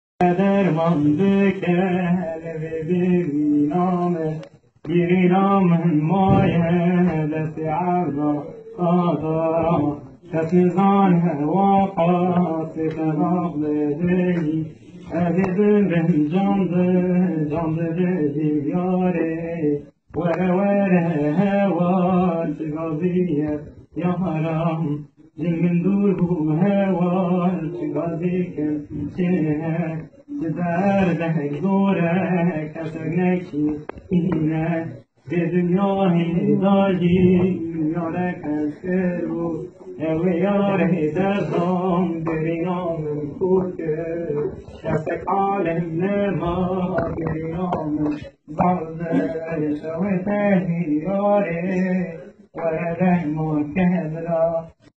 Ağrı Diyadin ilçesinde elektrik çarpması devamı⬇⬇
YÜREĞİMİZİ YAKTIN ÇOCUK!
Ağrı’nın Diyadin ilçesinde, 17 yaşındaki Ramazan tele takılan bir kuşu kurtaracağım derken elektrik çarpıyor ve 2 kolunu kaybediyor.Şimdi hastane odasında Kürtçe bir türkü var dilinde.😓😓